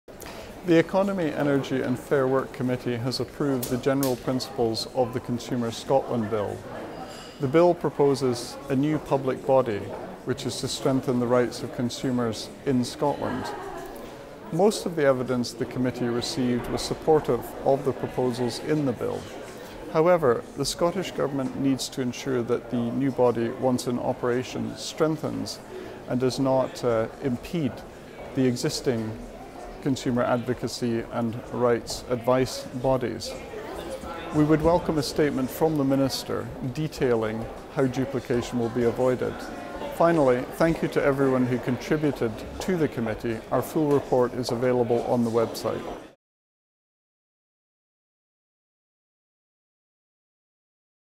Consumer Scotland Bill report
Gordon Lindhurst MSP, Convener of the Economy, Energy and Fair Work Committee, shares the key findings from the Committee’s report on the Scottish Government’s proposals to establish a new consumer rights body.